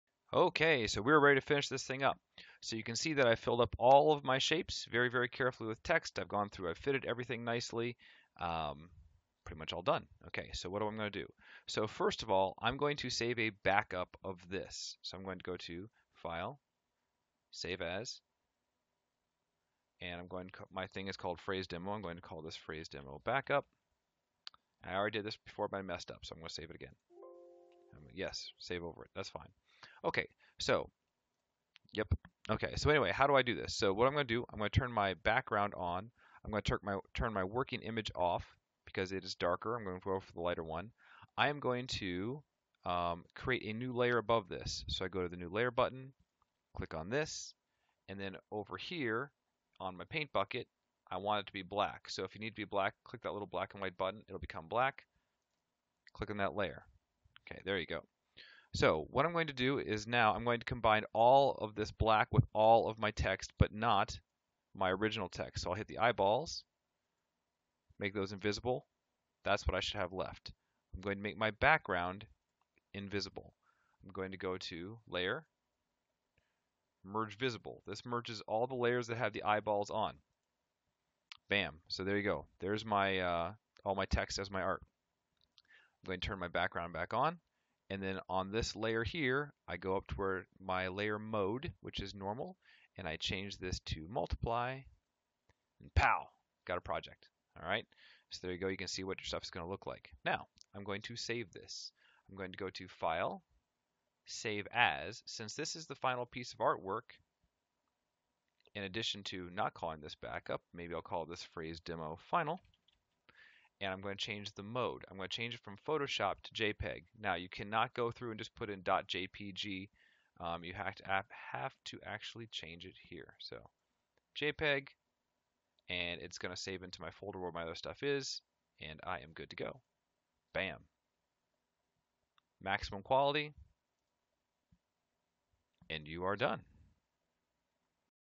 PHRASE 5 COMPLETION
This video show you how to save your project so that the photo shows through the text.